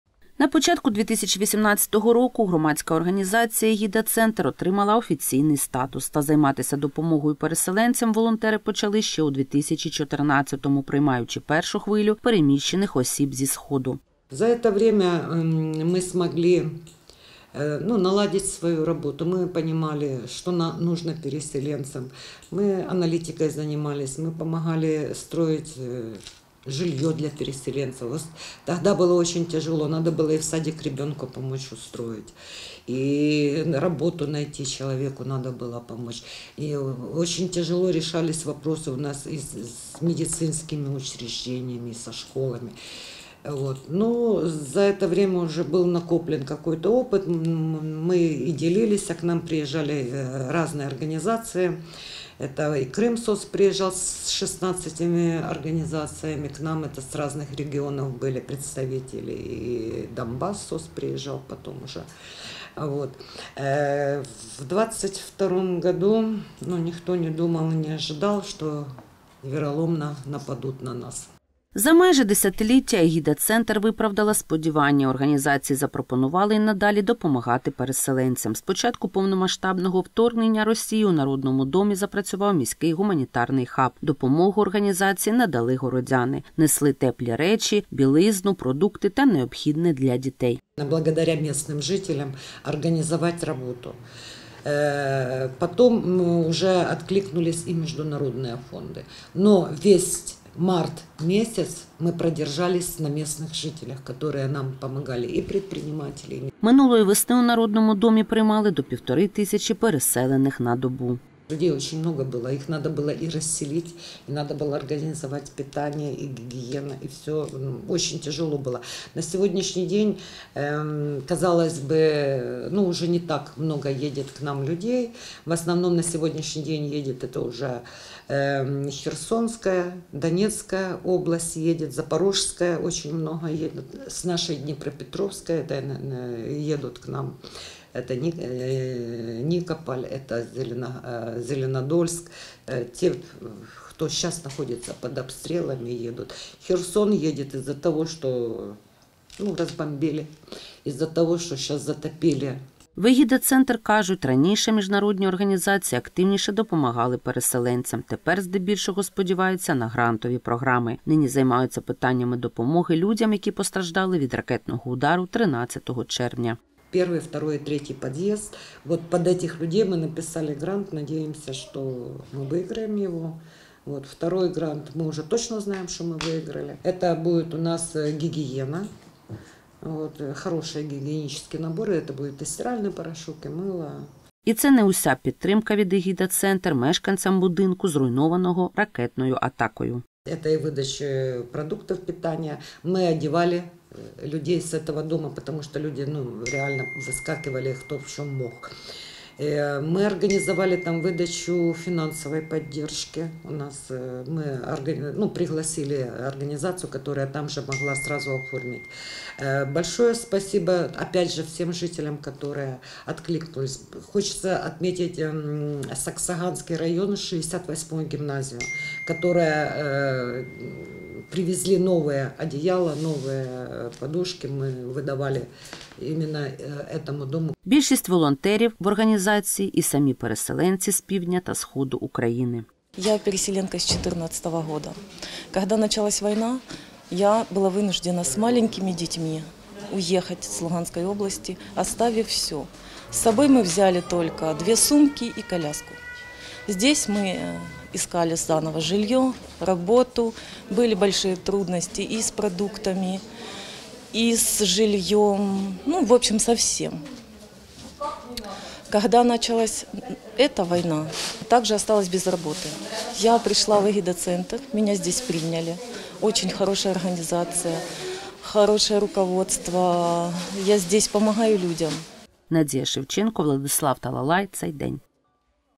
Підтримати в біді: волонтери ГО "Егіда-Центр" мають 9-річний досвід допомоги вимушеним переселенцям
20 червня більше як у ста країнах, у тому числі в Україні, відзначався Всесвітній день біженців. Для українців це болюча тема. Адже, починаючи з 2014 року і до сьогодні, кількість переміщених не зі своєї волі осіб збільшується. За офіційними даними, у Кривому Розі офіційно зареєстровано понад 74 тисячі вимушених переселенців. Реальна цифра - понад 80 тисяч мешканців сходу та півдня країни. Значний досвід у роботі з переселенцями напрацювала громадська організація "Егіда-центр". Через цей гуманітарний хаб прибули до нашого міста та переміщувались транзитом близько ста тисяч людей. Про роботу центру - в сюжеті Надії Шевченко.